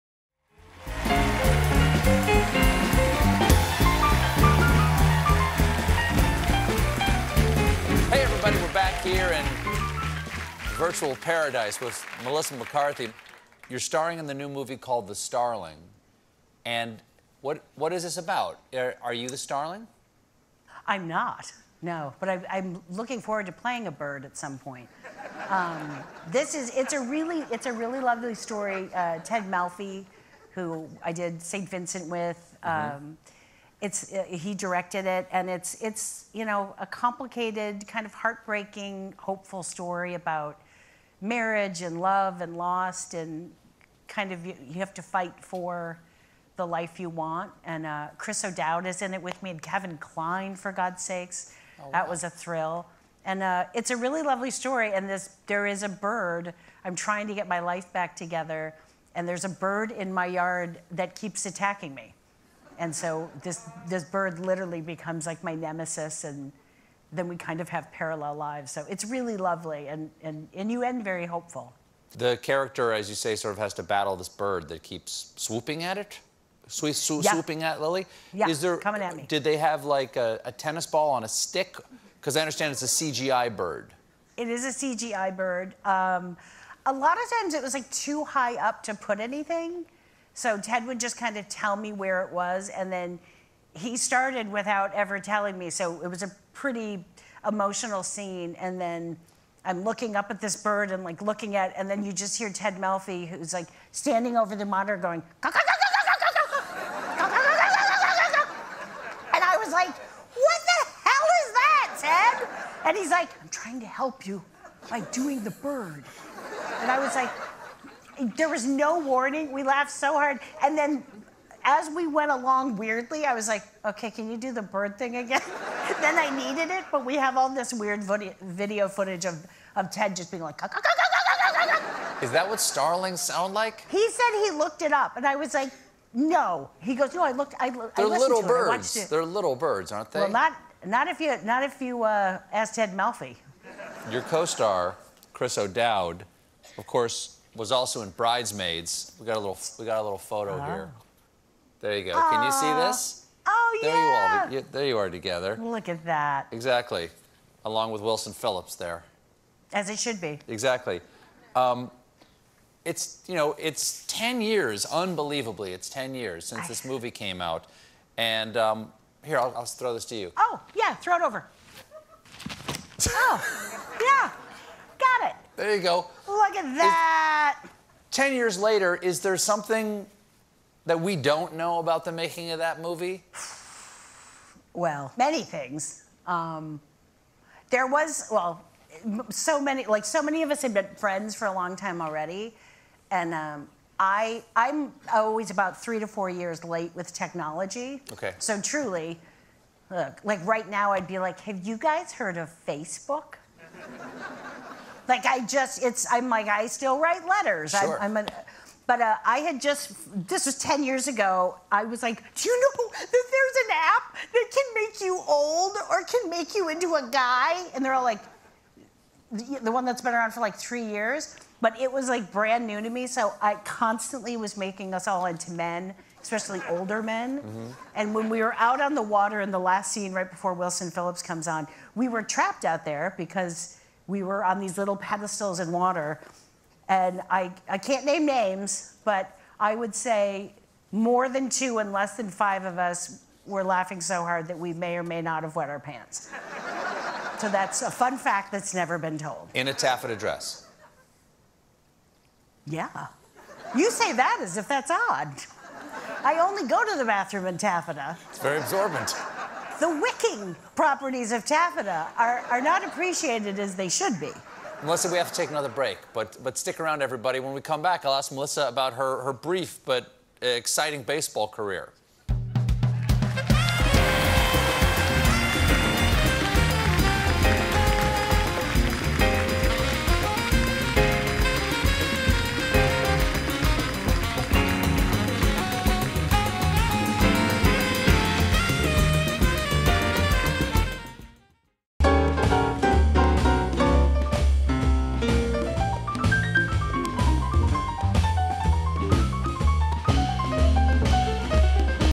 Melissa McCarthy On The Challenges Of Acting Opposite A CGI Bird In "The Starling"
In part two of Stephen's sit-down with Melissa McCarthy, she describes the unique situation she found herself in when acting alongside a computer-generated bird in her new film, "The Starling."